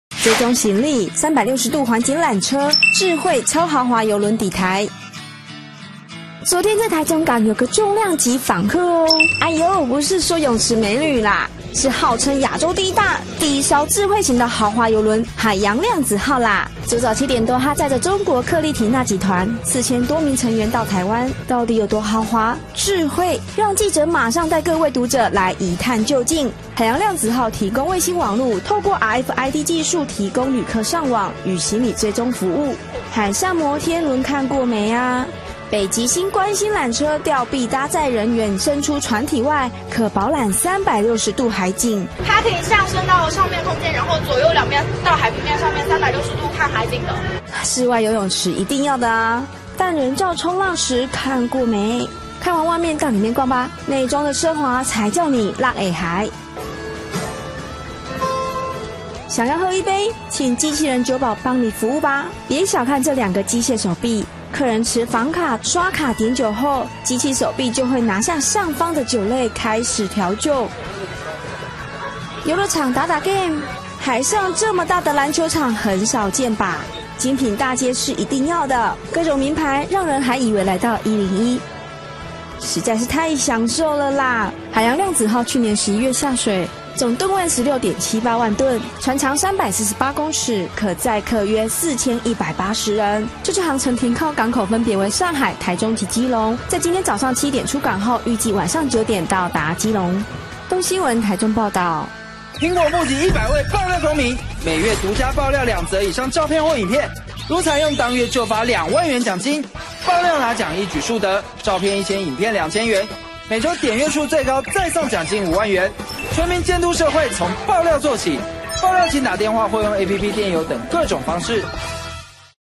追蹤行李 360度環景纜車 智慧超豪華郵輪抵台--蘋果日報20150903
【孫義方、王乙徹╱台中報導】亞洲最大豪華遊輪「海洋量子號（Quantum of the Seas）」，由中國克麗緹娜集團包船，昨搭載四千多人進入台中港碼頭停靠。中市府表示這種新型態的旅遊，每年可望為中部創造上億元觀光產值。但專家提醒，若僅提供遊輪靠泊，恐只增加遊客人次，對地方經濟助益有限。

追蹤行李 360度環景纜車
智慧超豪華郵輪抵台
昨天在台中港
有個重量級訪客
哀唷
不是說泳池美女
是號稱亞洲第一大
第 一艘智慧型的豪華郵輪
「海洋量子號」啦
昨早7點多
它載著
中國克麗緹娜集團
4000多名成員到台灣
到底有多豪華 智慧
讓記者馬上帶各位讀者
來一探究竟
「海洋量子號」提供衛星
網路
透過RFID技術提供旅客上網
與行李追蹤服務
海上摩天
輪看過沒啊
北極星觀景纜車
吊臂搭載人員伸出船體外
可飽
覽360度海景
它可以上升到上面空間
左右兩邊到海平
面上面
360度看海景的
室外游泳池一定要的啊
但人
造衝浪池看過沒
看完外面到裡面逛
內裝的奢華才是叫你掉下巴
想要喝一杯
請機器人酒保 幫你服務吧
別小看這兩個機械
手臂
客人持房卡刷卡點酒後
機械手臂就會拿下
上方的酒類
開始調酒
遊樂場打打GAME
海上這麼大的籃球場很少見吧
精 品大街是一定要的
各種名牌
讓人還以為來到101
實在是太享受了啦
「海洋量子號」去年11月下水
總噸位16.78萬噸
船長 348公尺
可載客約4180人
這次航程停靠港口
分別為上海、 台中及基隆
在今天(9/3)早上7點出港後
預計晚上9點到達基隆
動新聞 台中報導